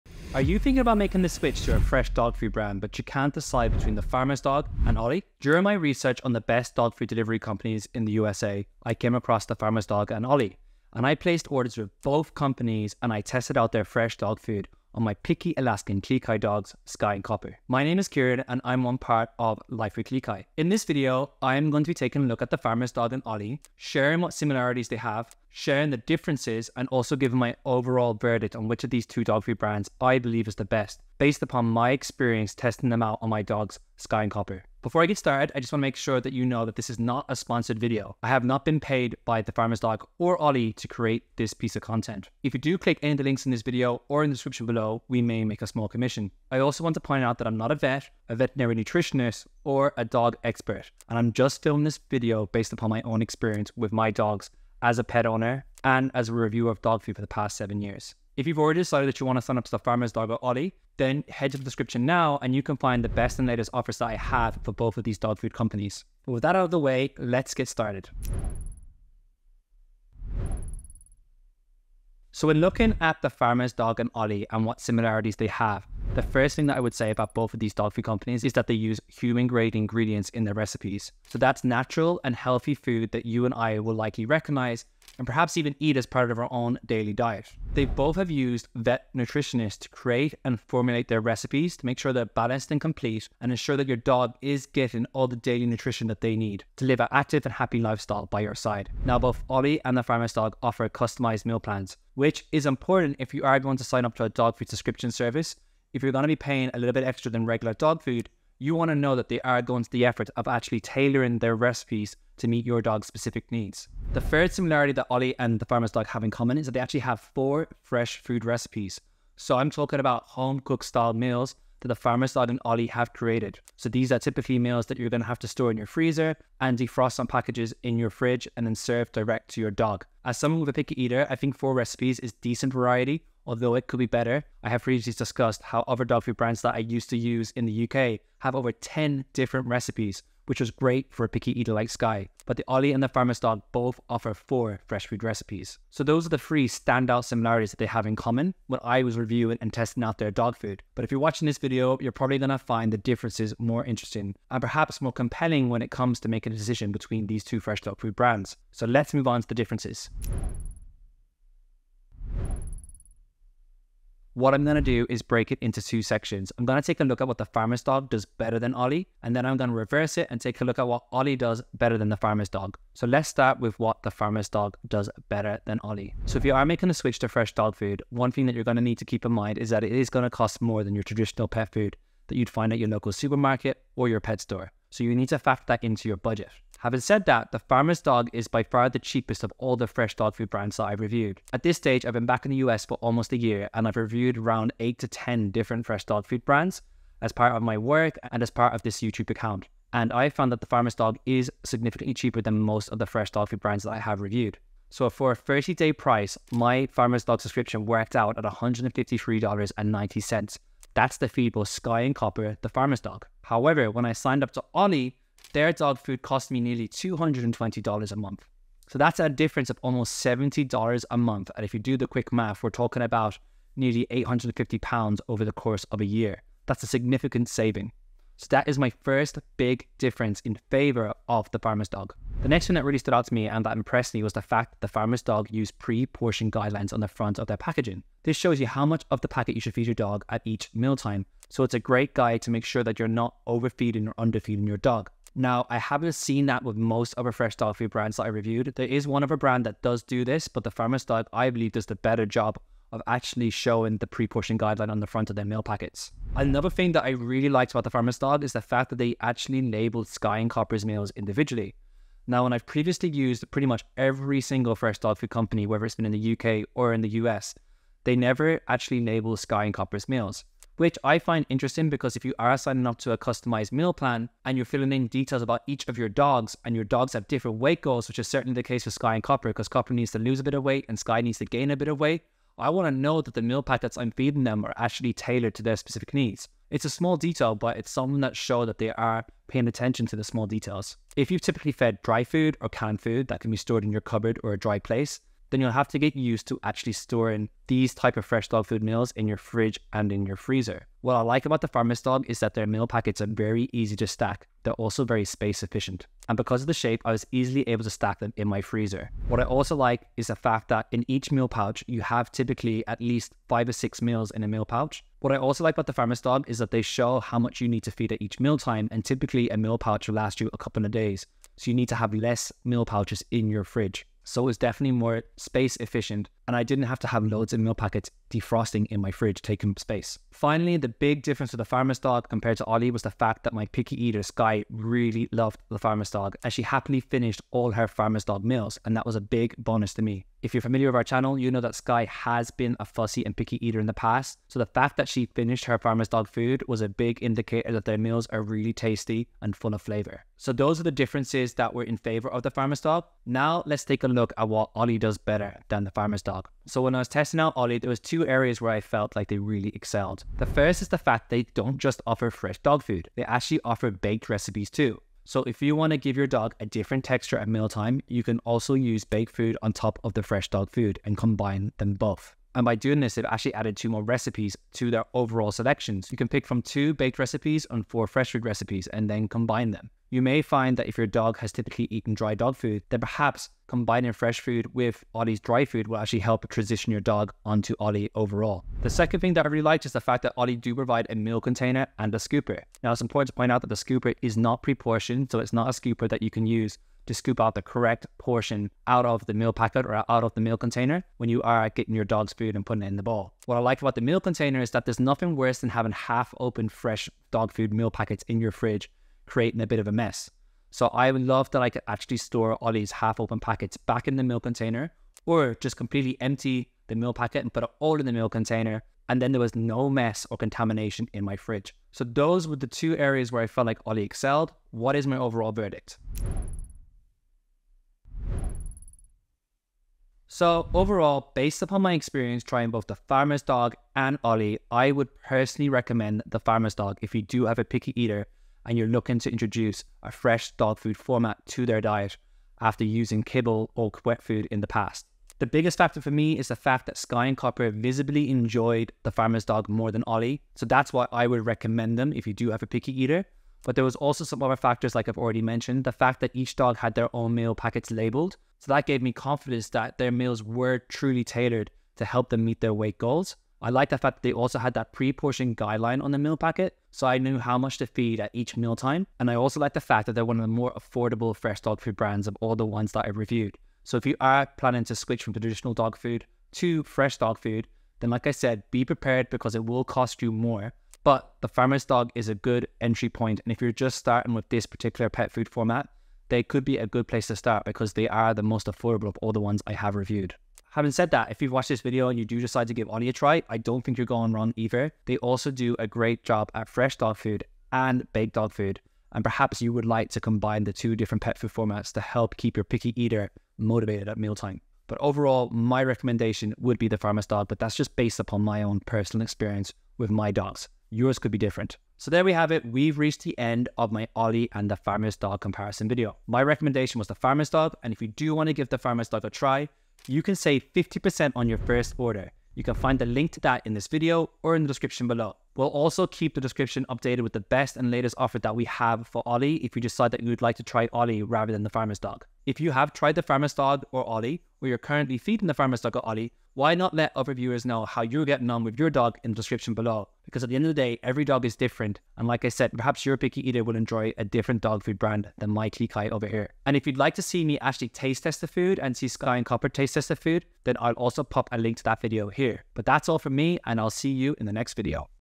The Farmer's Dog Vs Ollie: Which Is The Best?
Save 50% On The Farmer's Dog

Save 50% On Ollie Dog Food

Recap of the video:

The Farmer's Dog

When we first got Copper and Skye in 2017, we fed Nom Nom before moving to the UK and signing up to Butternut Box. While the Farmer’s Dog wasn’t on my radar six years ago, they were created in 2015 so they’ve been steadily improving their business.

I was intrigued to try The Farmer’s Dog after seeing so many sponsored posts and positive reviews on social media of their fresh dog food.

Signing up to The Farmer’s Dog was really straightforward and they’ve got an easy-on-the-eye sign-up questionnaire when compared to other pet food brands operating in the fresh food marketplace.

I liked that The Farmer’s Dog have lots of information about their food as you complete the questionnaire. They use high quality meat and human grade ingredients formulated by veterinary nutritionists.

It felt good reading that The Farmer’s Dog doesn’t use any additives, nasties or preservatives. There’s no meat meals, brown pellets or mislabelled recipes. Just fresh, healthy ingredients.

When I received my first The Farmer’s Dog delivery, I really liked the family feel to the box. It contained a pamphlet with more information about their fresh food plus a cooler bag.

Most importantly, The Farmer’s Dog meals were frozen rather than thawed. Having received multiple deliveries from this dog food brands, I haven’t had any issues with The Farmer’s Dog.

I was really impressed with the vibrancy of the meals – there’s lot of colorful vegetables plus healthy-sized chunks of meat.

The Farmer’s Dog do standout from all the other fresh dog food delivery brands that I’ve tried when it comes to the pre-portioning element.

Whether you’ve got one dog or two like me, The Farmer’s Dog have a helpful graphic on the front of each meal pouch to display how much to feed Copper and Skye at mealtime.

This makes their meals truly pre-portioned unlike some other fresh pet food brands.

I think The Farmer’s Dog are competitively priced when compared to their competitors in the fresh frozen meal marketplace.

Ollie:

I was aware of Ollie when I first searched for fresh dog food meals back in 2017 – but ultimately decided to go with Nom Nom because of a recommendation from a dog owner.

They’ve been growing since I first heard of them and I was impressed some of the improvements made – such as the pet food container and scooper.

Ollie’s sign up questionnaire is pretty simple, but could be a little smoother to use.

They’ve got a good range of recipes to cater to pet parents with a fussy eater like Skye. As a dog owner with a fussy eater, I also appreciate the ability to mix fresh food recipes.

While I thought the container and scooper was a thoughtful inclusion, I was disappointed that Ollie’s packaging didn’t appear to be pre-portioned like The Farmer’s Dog.

Just to look at, Ollie’s dog food didn’t look overly appetizing. It was more like a slab of pâté rather than an enticing, fresh dog food meal. Ollie food packs had the shape of a brick rather than pet food.

When it came to feeding Ollie to Copper and Skye, it was hit and miss. Copper showed a lot of interest at the start but as the weeks went by, he started to show less interest.

Skye wasn’t really a fan of Ollie and she was slow to go to her food bowl and even slower to finish her meals. At the start, Copper was happy because he got leftovers but eventually he barely emptied his bowl.

When compared to other pet food brands who offer frozen meals, I don’t consider Ollie to be extortionately expensive but they do carry a premium price tag.